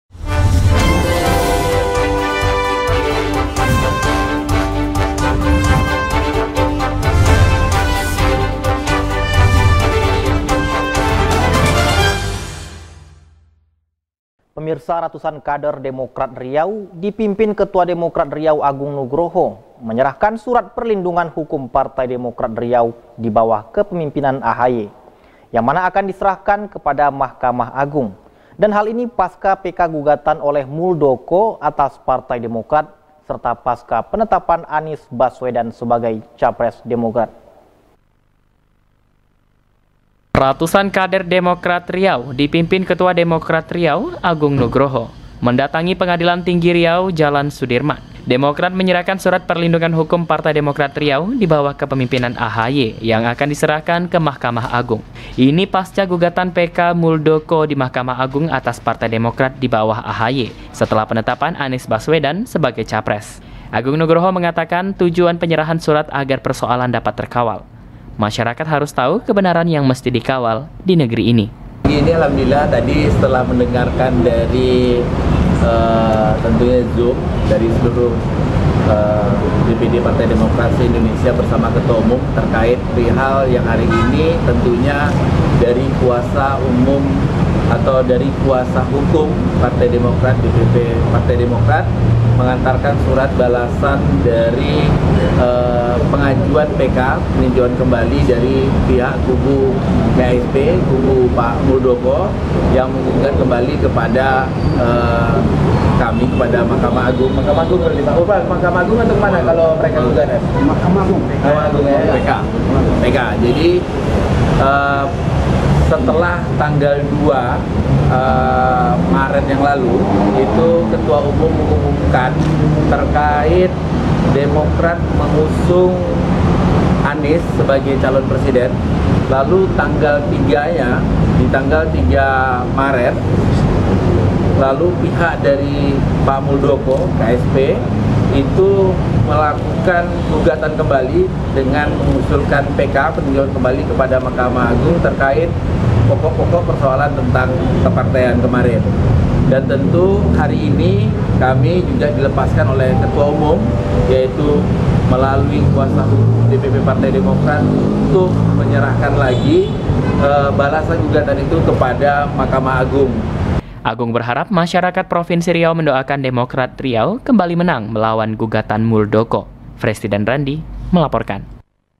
RATUSAN KADER DEMOKRAT RIAU SERAHKAN SURAT PERLINDUNGAN HUKUM
Pekanbaru(Riautv)- Ratusan kader Demokrat Riau, dipimpin Ketua Demokrat Riau, Agung Nugroho, menyerahkan surat perlindungan hukum partai Demokrat Riau, dibawah kepempimpinan AHY, yang akan diserahkan ke Mahkamah Agung. Hal ini pasca PK gugatan oleh Muldoko atas partai Demokrat, pasca penetapan Anies Baswedan sebagai capres Demokrat.